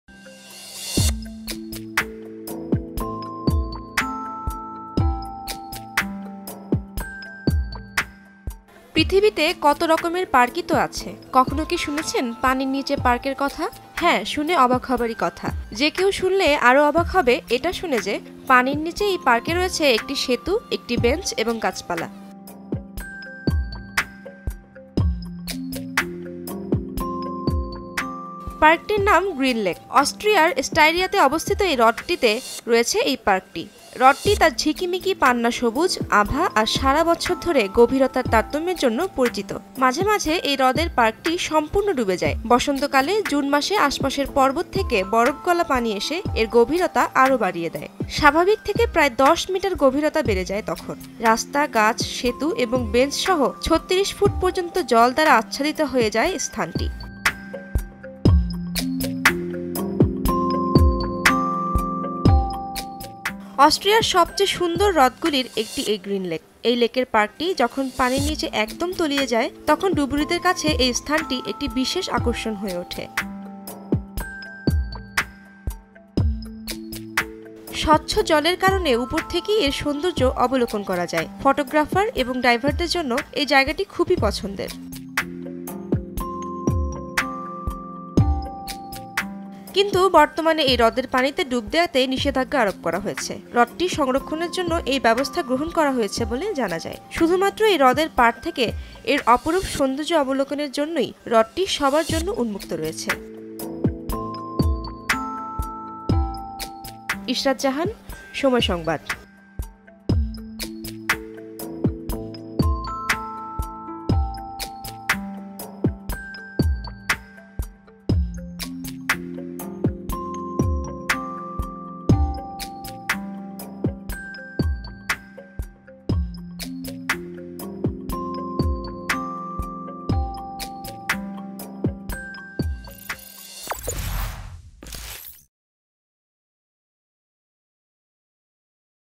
অস্ট্রিয়ার অবিশ্বাস্য আন্ডারওয়াটার পার্ক | Underwater Park | Gruner Lake | Somoy TV
পৃথিবীতে কত রকমের পার্ক ই তো আছে। কখনও কি শুনেছেন পানির নিচে পার্কের কথা। হ্যা শুনে অবাক হবারই কথা। যে কেউ শুনলে আরও অবাক হবে এটা শুনে যে পানির নিচের এই পার্কে রয়েছে একটি সেতু, একটি বেঞ্চ এবং গাছপালা।

There are so many types of parks in the world. Have you ever heard of an underwater park? Yes, I should be surprised to hear. Anyone would be even more surprised to hear that this underwater park has a bridge, a bench and plants.